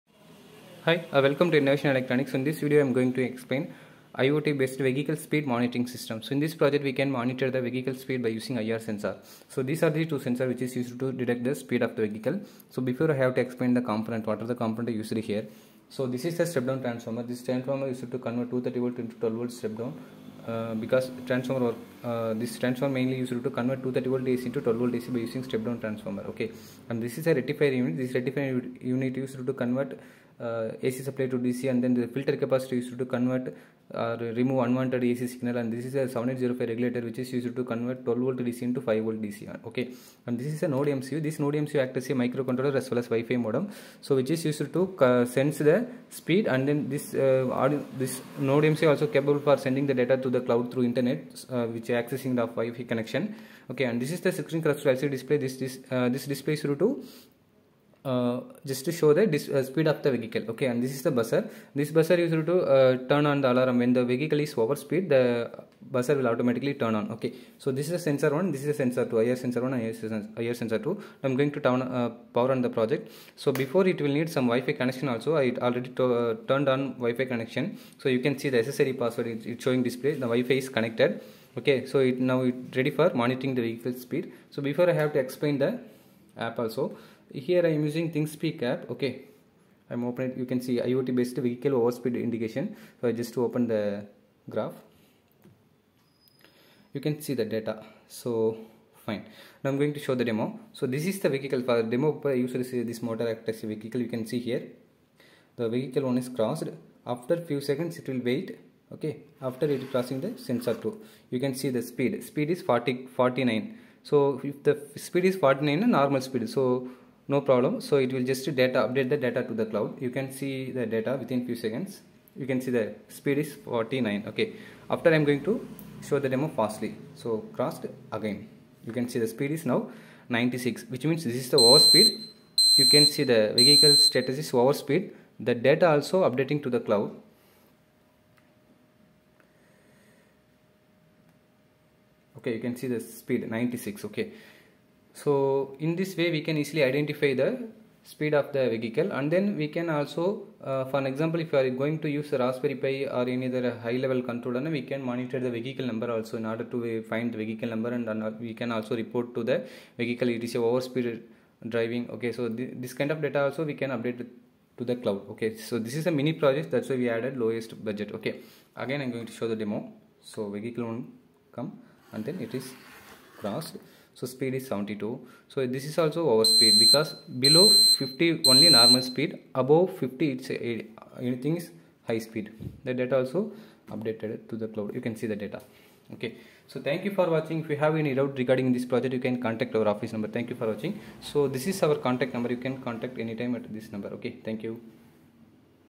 Car Speed Detector using Arduino with IR Sensor
Every country has different rules for driving cars on the roads. But the most common and important rule in any country is the vehicle speed limit on particular roads. Because a high-speed vehicle can happen a road accident and can make huge damage. For this reason, the patrolling officers are using a car speed radar gun and check car speed manually. This is very difficult to check one by one vehicle speed for the patrolling officers.

That’s why we will make the Car Speed Detection | Car Speed Measurement Device with Over Speed Indicator project using Arduino and IR Sensor. We can be placed this device beside the road. Which can measure the vehicle speed automatically and show the vehicle speed on a display. This device has one more feature, when any vehicle crosses the speed limit then it indicates by a Red light.

INNOVOTRONICS
Sudhakar Nagar,
(opp Crompton service center) Villupuram,